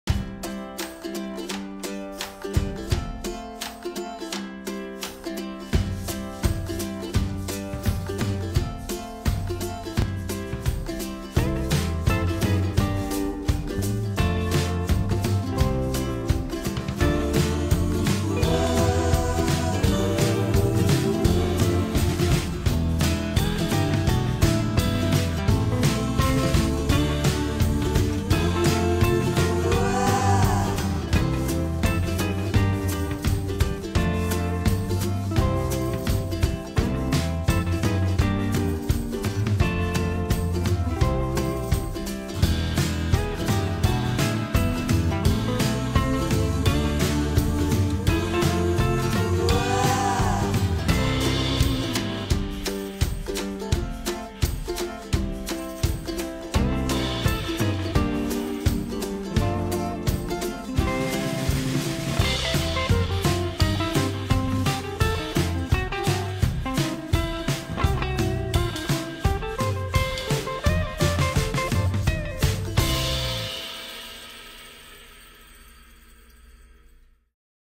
フレンチ・ブルドッグ ロイくん【French bulldog】
甘えん坊さんなロイくん♪
撫でて！とお腹を向けてくれる姿が可愛いです☺️